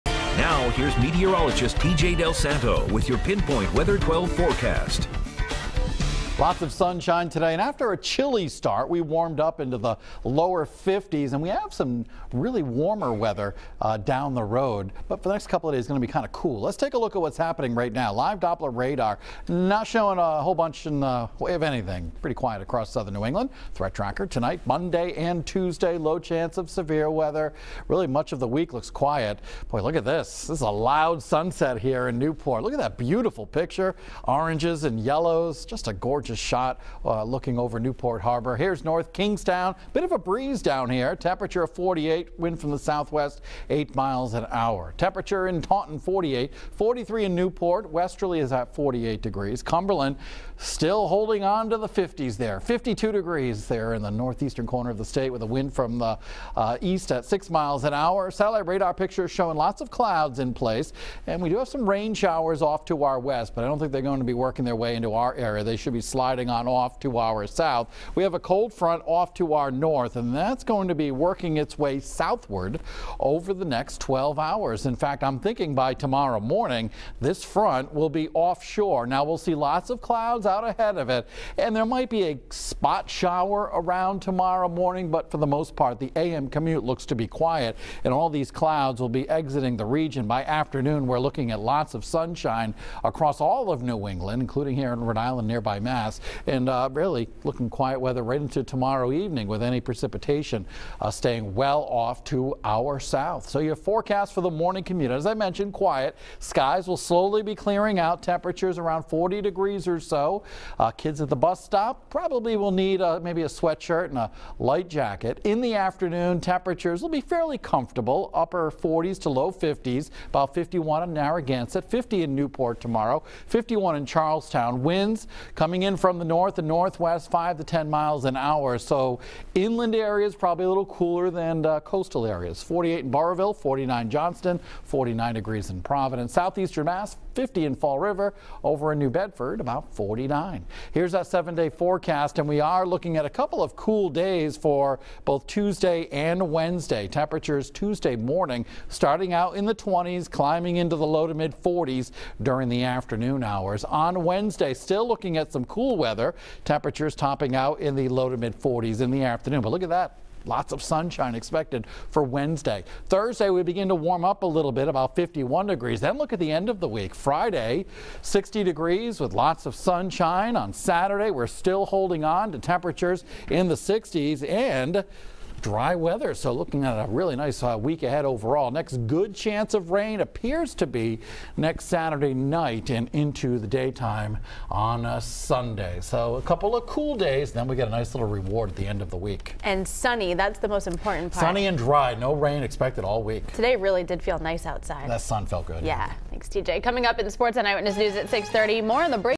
Pinpoint Weather 12 Forecast
T.J. Del Santo's Pinpoint Weather 12 Forecast from 3/24/2019.  Mainly dry weather for awhile.